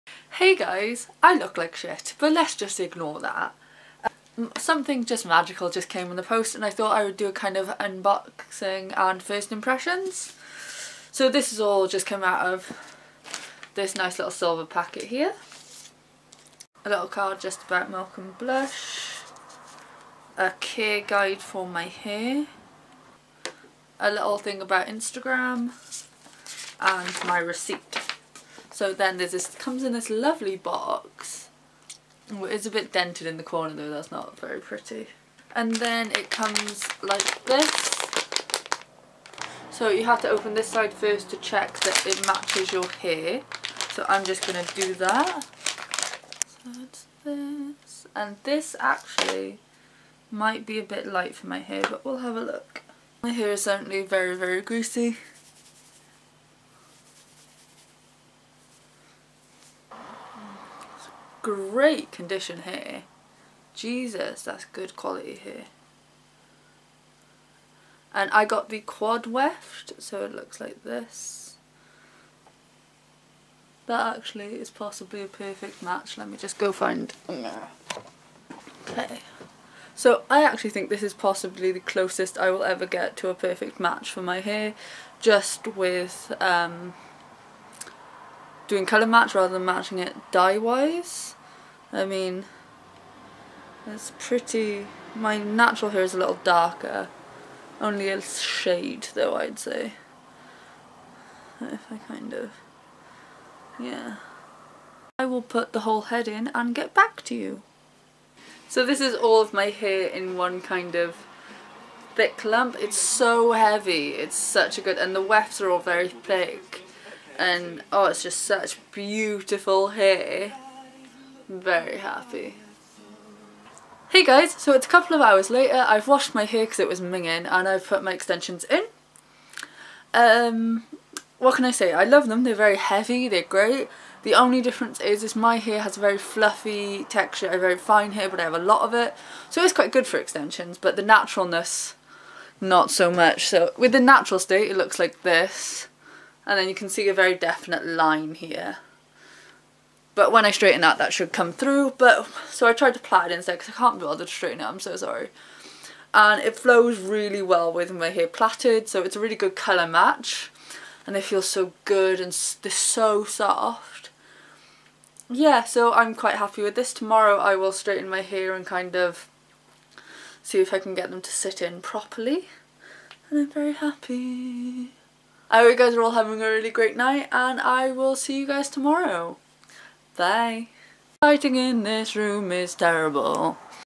VLOGMAS! Dirty Looks Unboxing + First Impressions | Rhiannon Emily
Vlogmas day 8! So today my hair extensions from Dirty Looks arrived so I thought I'd film an unboxing and first impressions!

I love hair extensions but I haven't worn them since I had red hair and I miss them greatly so I thought I'd splash out.
I bought the colour "with a tan"